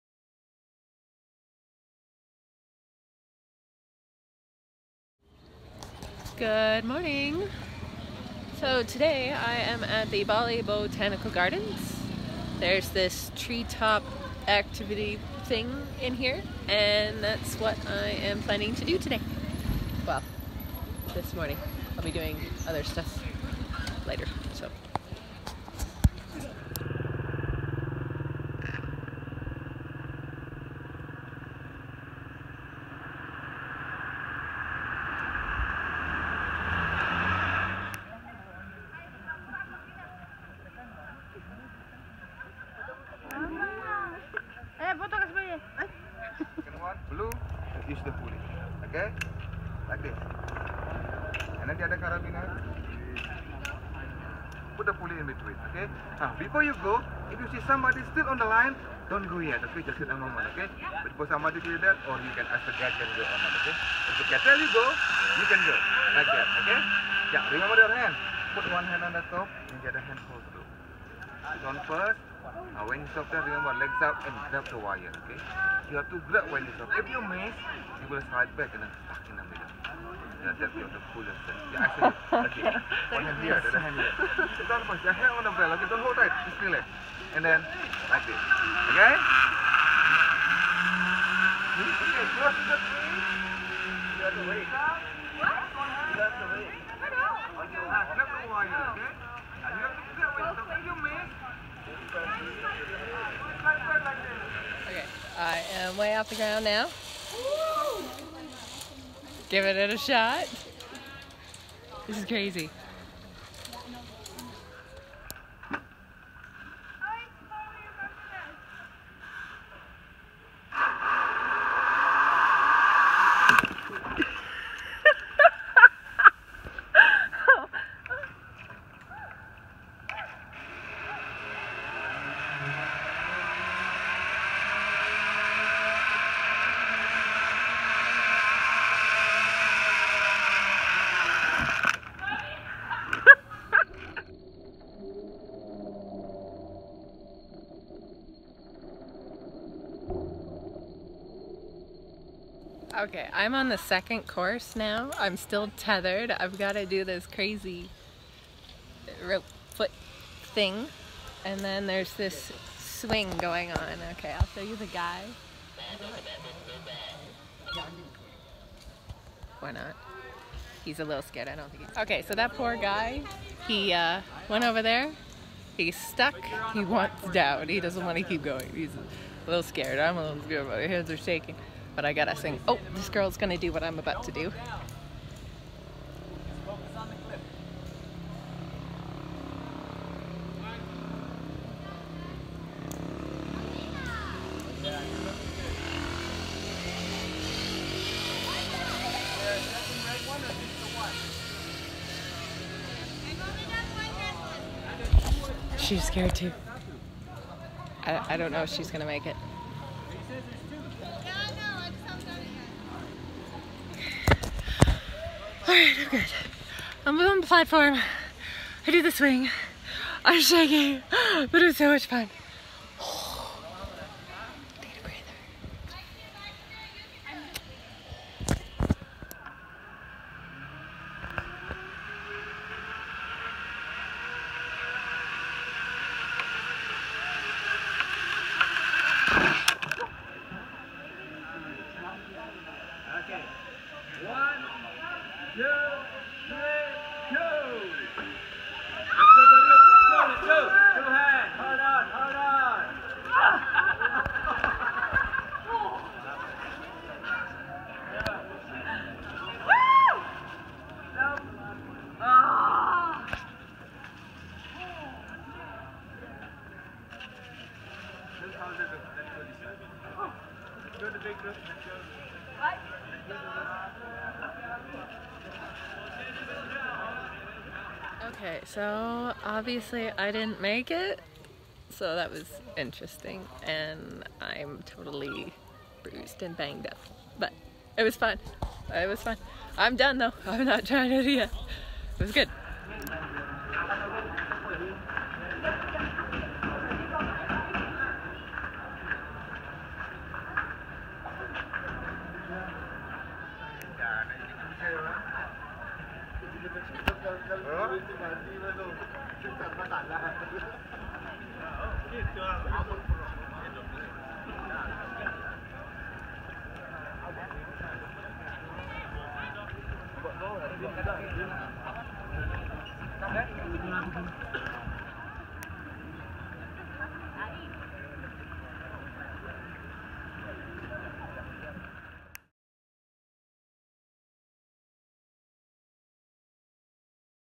Bali Vlog Day 8 - North Bali
Wednesday April 4 I had planned to do the Mount Batur sunrise hike but found that I was more tired than I thought I would be, so I stayed more local. I rented another scooter and headed to Bali Botanic Garden and had fun on the Bali Treetop Adventure Park courses. I then went to Pura Danu Beratan until it started raining and enjoyed some lunch at a nearby restaurant. I went to bed early to prepare for the next days sunrise activity. The homestay I stayed at was again the Wanagiri Ecolodge found on Airbnb And remember, if you use my link (below) and sign up for Airbnb you get a $45 CAD travel credit.

Hotels Featured
Homestay
Wanagiri Ecolodge
Found on Airbnb

Places Visited
Bali Botanic Garden